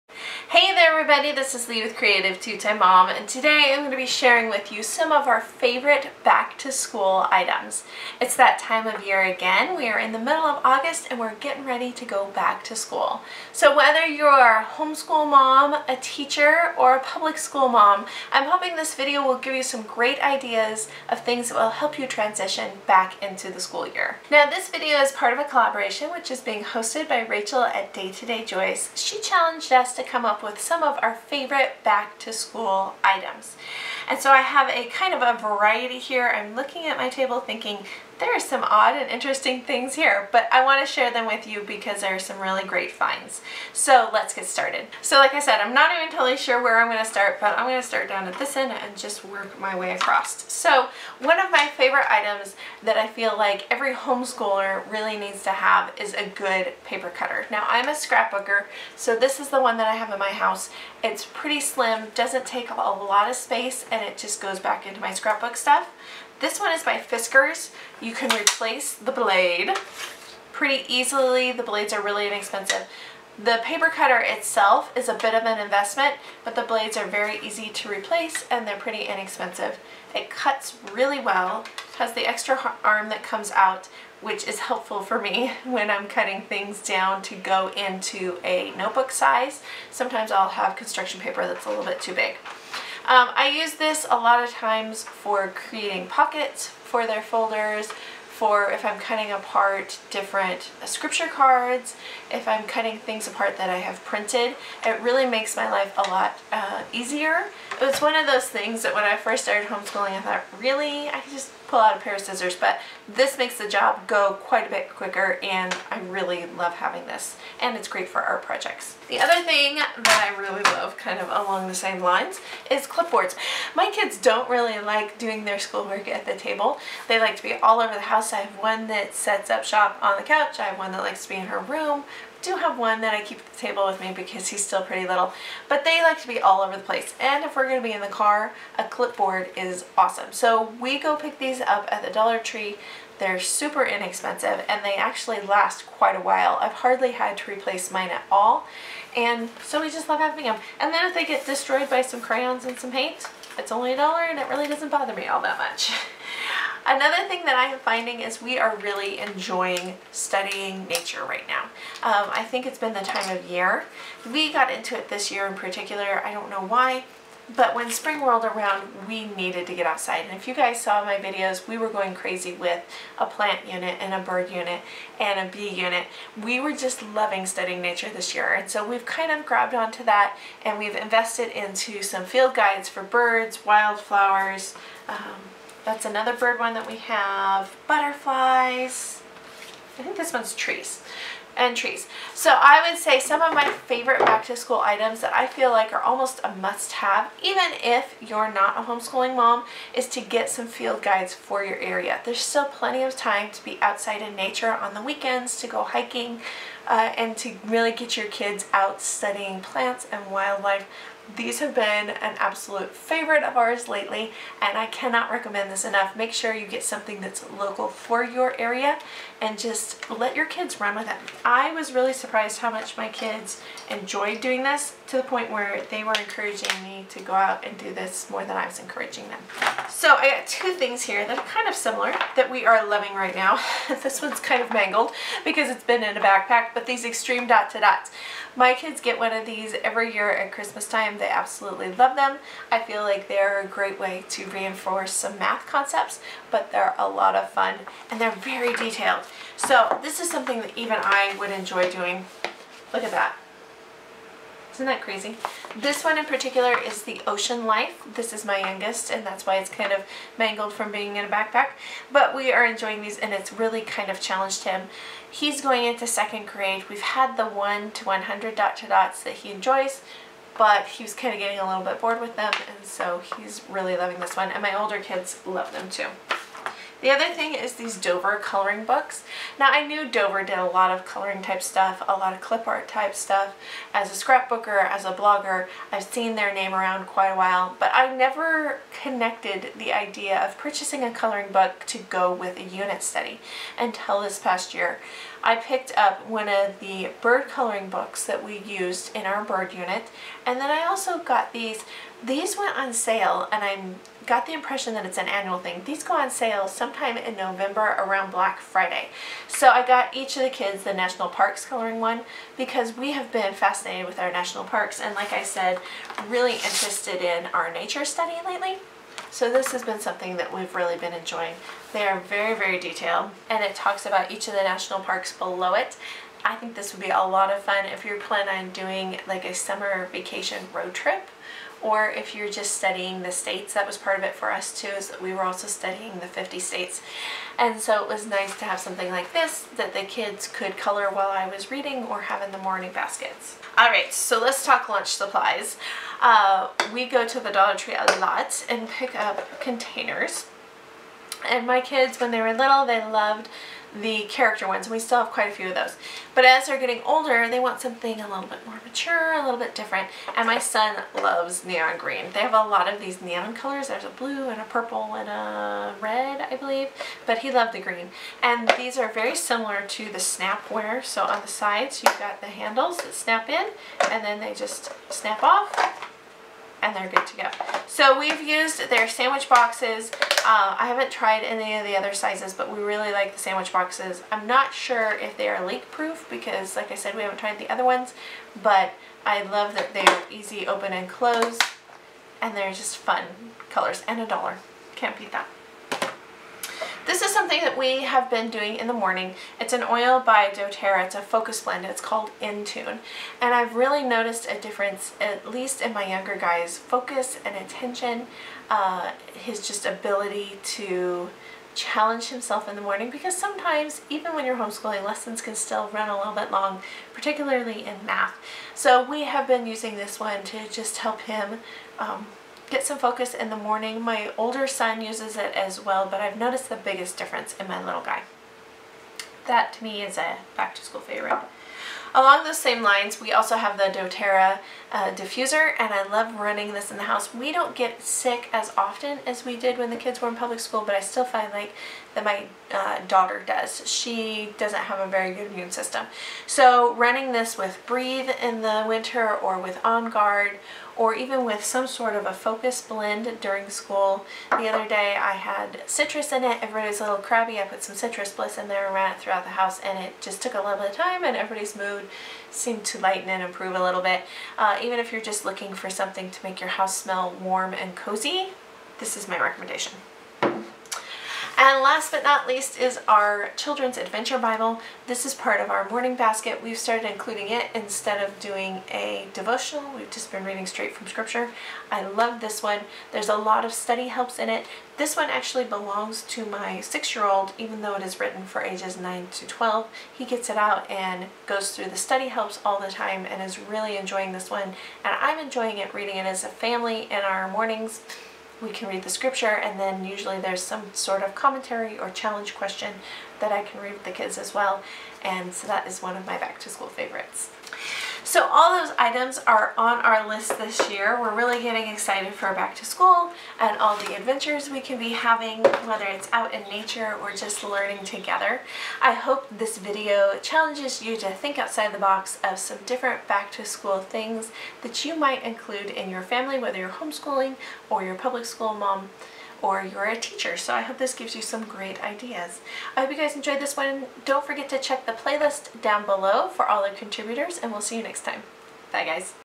Back to School Favorites ll Collab
Today I'm sharing some of our favorites for going Back to School!

Join The Creative2xmom Family on their journey of being a family of 5! 💑16yrs led to 👱12 yrs, 👧10 yrs, and 👱 6 yrs.